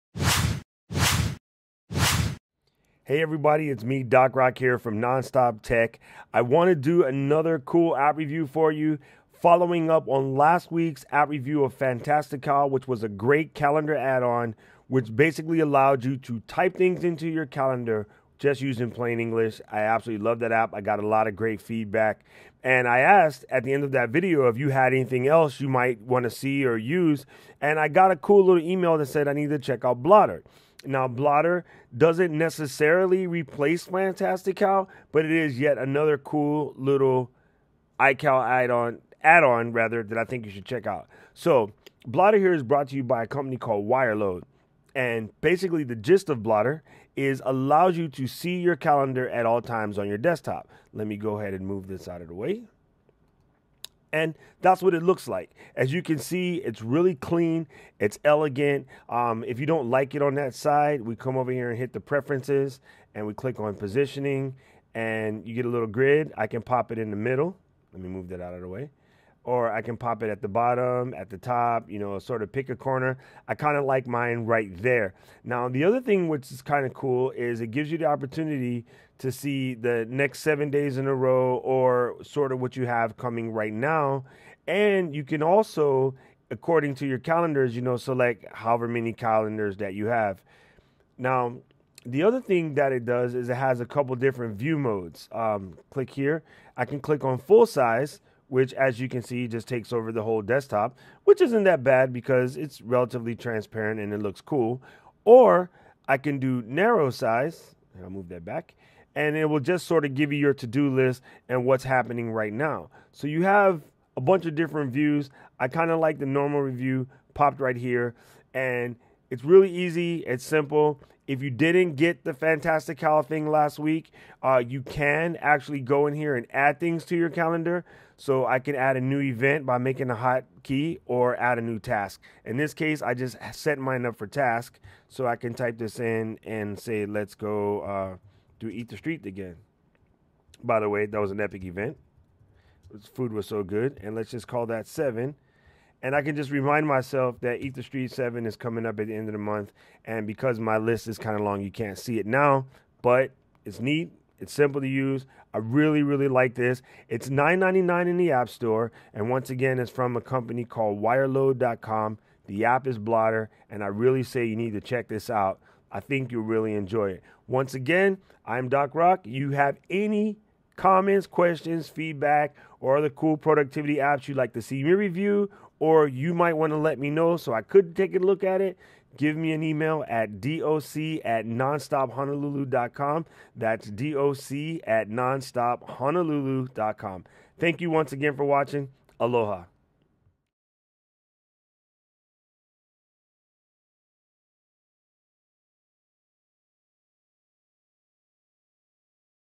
Blotter Review
Blotter is a desktop calendar viewer for the Mac. It allows an "Always On" look at your iCal and Google calendars with out the need to open their respective programs. Blotter does allow for quick entry for Events and Task so you can even add a quick to-do and dinner party without opening your calendar.

and cost $9.99 in the MAS.